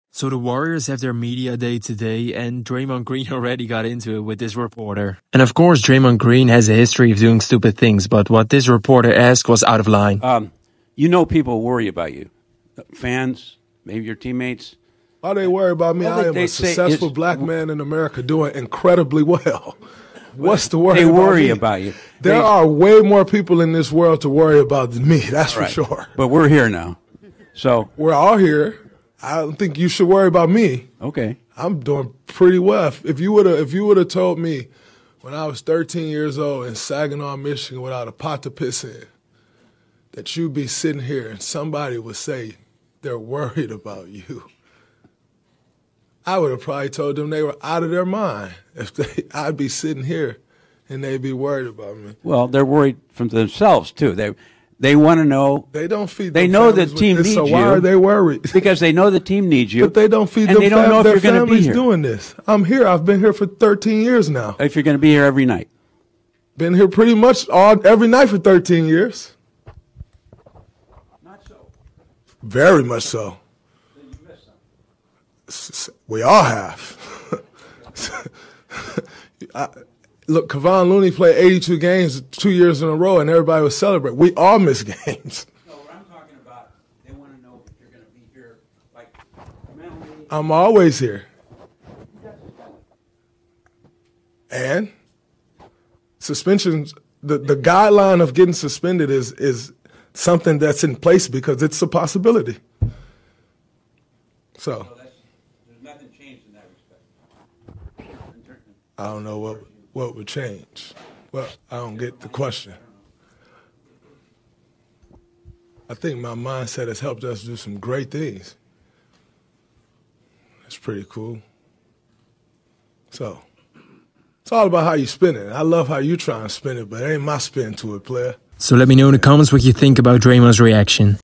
Draymond Green gets heated at reporter saying people are worried about him 😳
Draymond Green got into a heated back and forth with a reporter after he told Draymond that people were worried about him.